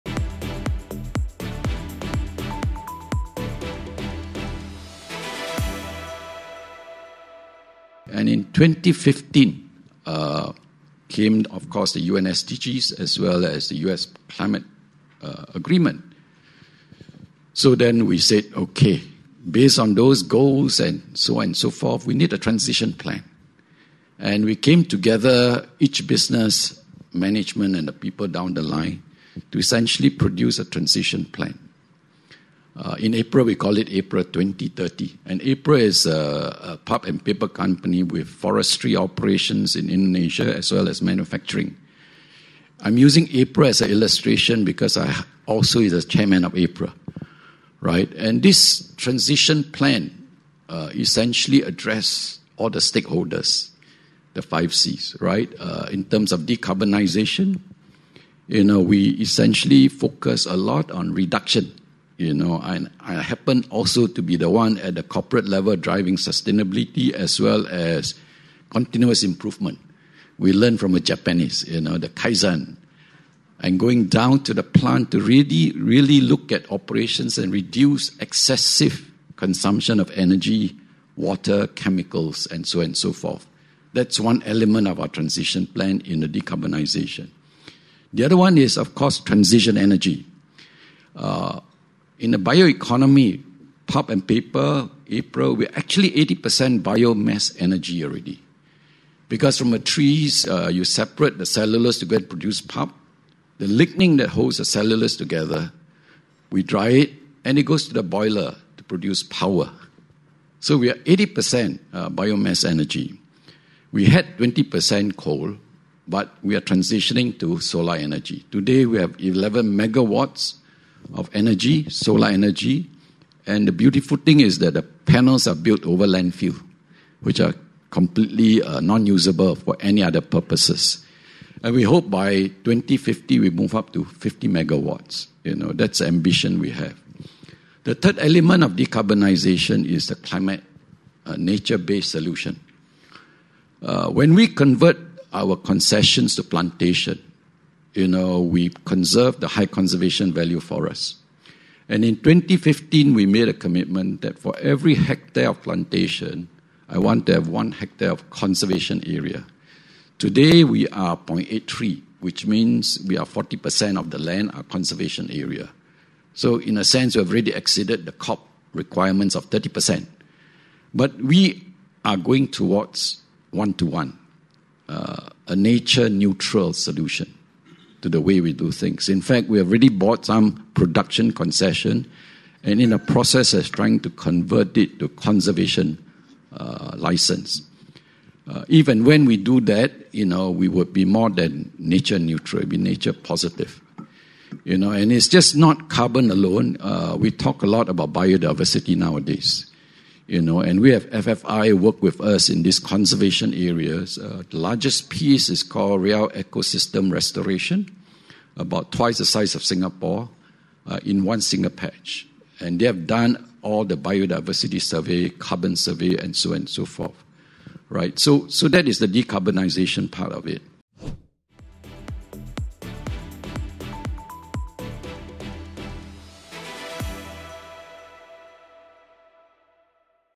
Share how Royal Golden Eagle executed its decarbonisation plans
Soo Khiang Bey Vice Chairman RGE, explains how the company executed its decarbonisation plans.

Bey was speaking at an event organized by The Asset Events.